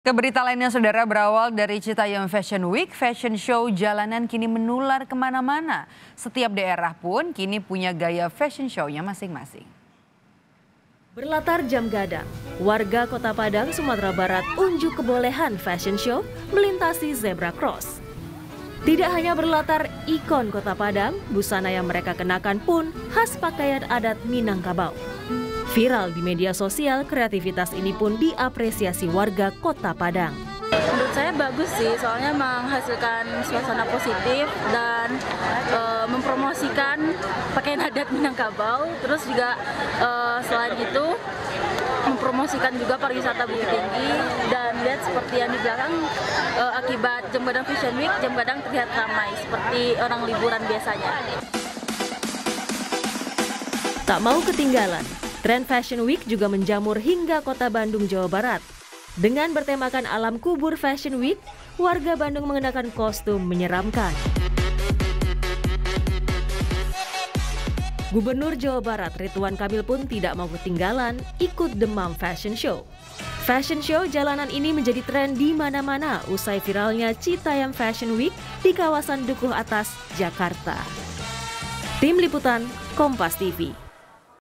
Demam Citayam Fashion Week Menjalar ke Padang hingga Bandung
KOMPAS.TV - Berawal dari Citayam Fashion Week, fashion show jalanan kini menular ke mana-mana.

Setiap daerah pun punya gaya peragaan busananya masing-masing.

Berlatar Jam Gadang, warga Kota Padang, Sumatera Barat, unjuk kebolehan fashion show melintasi zebra cross.

Baca Juga Viral Jam Gadang Fashion Week, Ajang Pamer Pakaian Adat Minangkabau di

Tidak hanya berlatar ikon Kota Padang, busana yang mereka kenakan pun khas pakaian adat Minangkabau.

Tak mau ketinggalan, tren fashion week juga menjamur hingga Kota Bandung, Jawa Barat.

Dengan bertemakan Alam Kubur Fashion Week, warga Bandung mengenakan kostum menyeramkan.

Gubernur Jawa Barat Ridwan Kamil pun tidak mau ketinggalan dan ikut dalam demam fashion show ini.